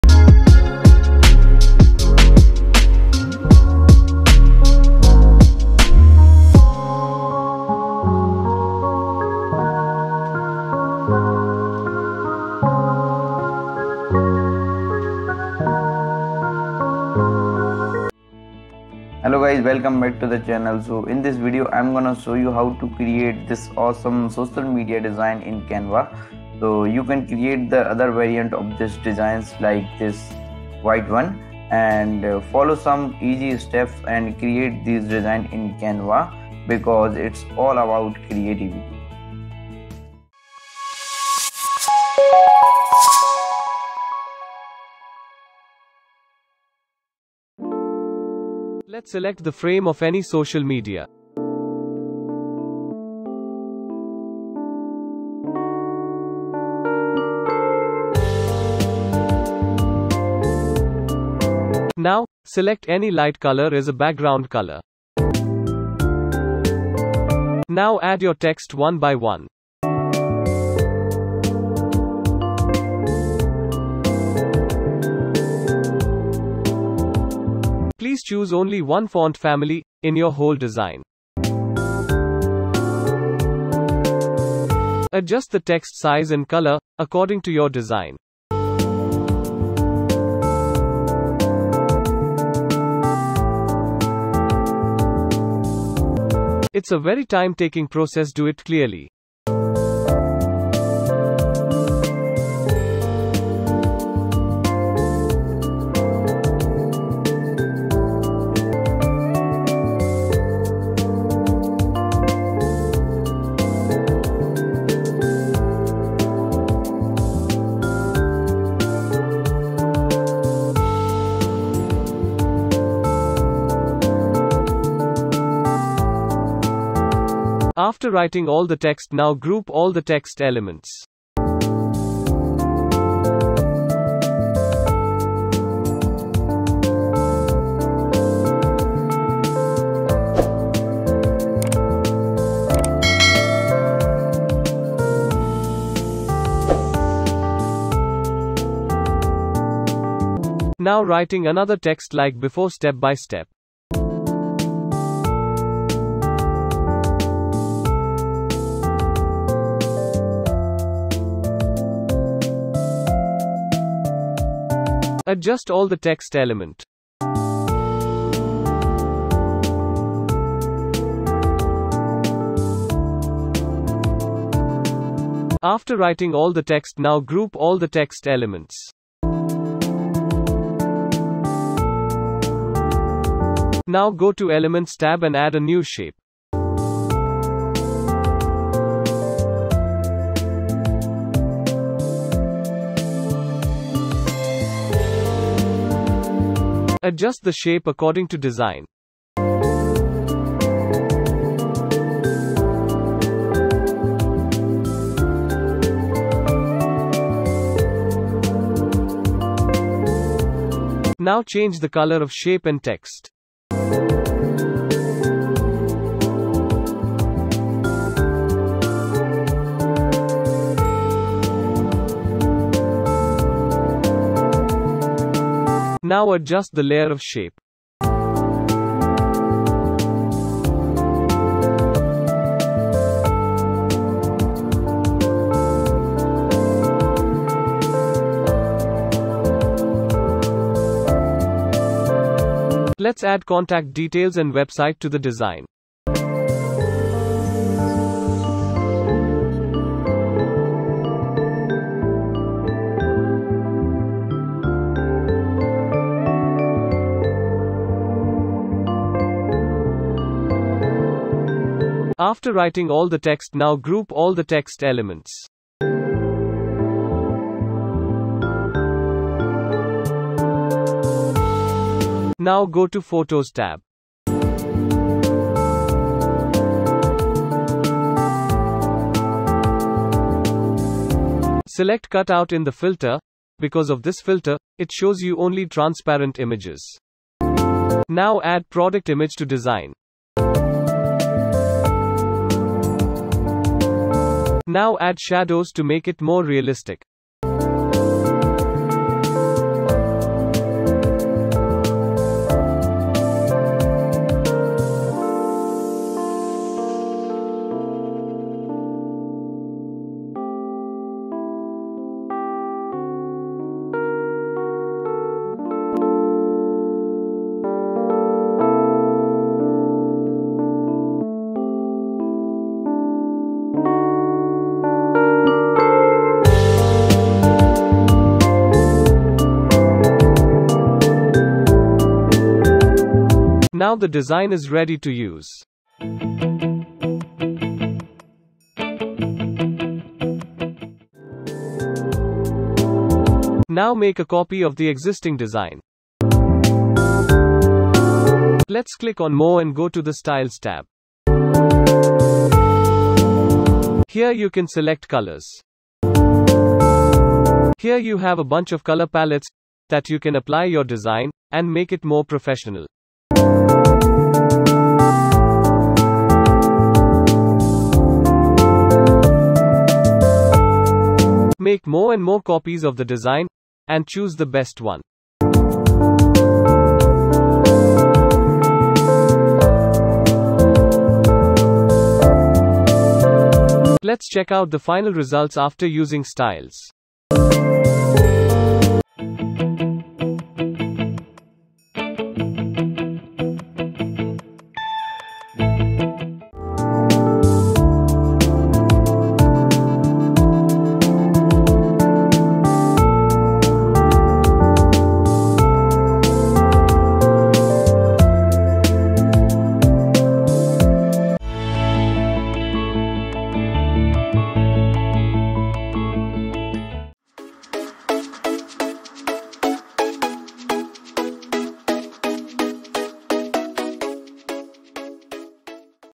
| Social Media Banner Design| Instagram post design in Canva | Tutorial | Designtalk | Part 5 |
In this video, I'm gonna show you how to create social media ad post in Canva so, watch this full tutorial.

DESIGN TALK - It's All About Creativity